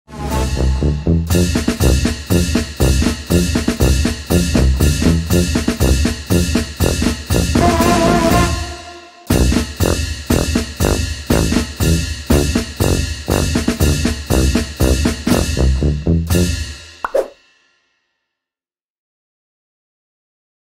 Sesión de fotos de cobayos para el Año Nuevo en Japón|CCTV Español
Para dar la bienvenida al Año de la Rata, el zoológico y jardín botánico de Kumamoto en Japón celebró recientemente una sesión de fotos de cobayos.